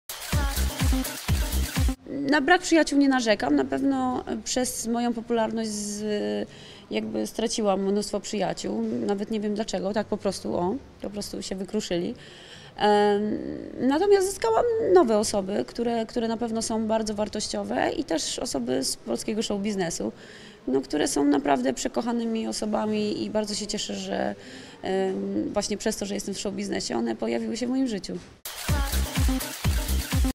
Siwiec: "Przez moją popularność straciłam mnóstwo przyjaciół!"
Natalia Siwiec w show biznesie pojawiła się cztery lata temu. Od tamtego czasu sporo się zmieniło. Natalia nie chce być modelką, która rozbiera się za pieniądze. Zamierza zostać poważną aktorką. Żali się, że od kiedy jest popularna, straciła mnóstwo przyjaciół.